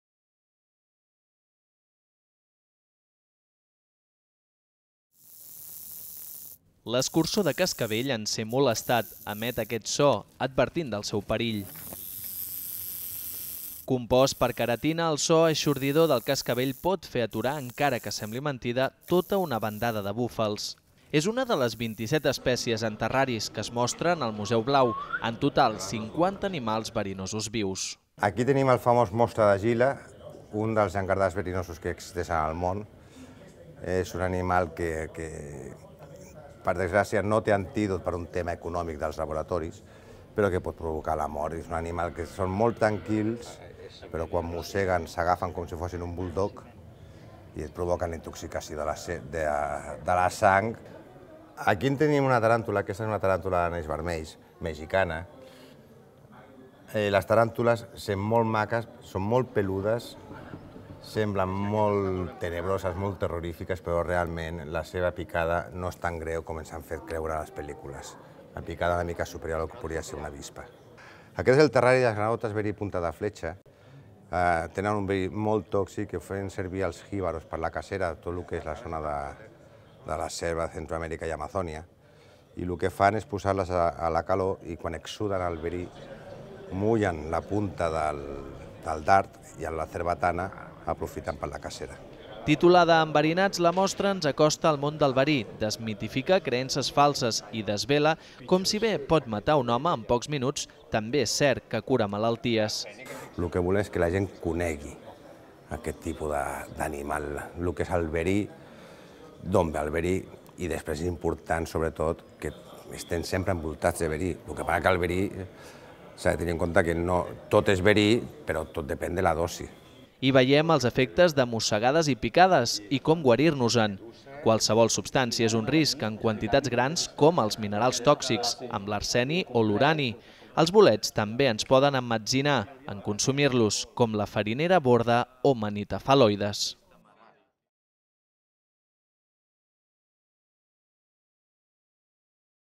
Animals verinosos vius, bolets i minerals tòxics, al Museu Blau
Fins l'1 de març de 2015, el públic es pot acostar al món del verí a la natura. S'hi mostra com si bé la substància tòxica pot provocar la mort de l'ésser humà en pocs minuts també pot curar-lo. Hi veiem rèptils, amfibis o plantes i minerals tòxics.